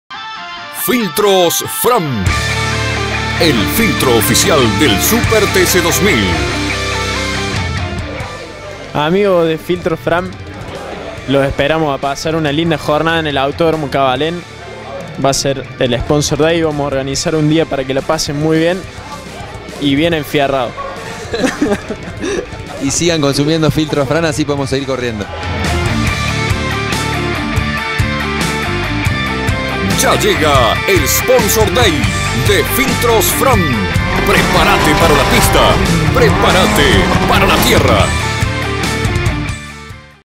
capsula 3 Filtros Fram Sponsor Day Copete Pilotos
Ya llega el nuevo SPONSOR DAY DE FILTROS FRAM...asi te lo cuentan los Pilotos de Renault Sport. del Ambrogio Racing. Se comparte con el VRS del Coyote Villagra en el Cabaalén Cordoobés.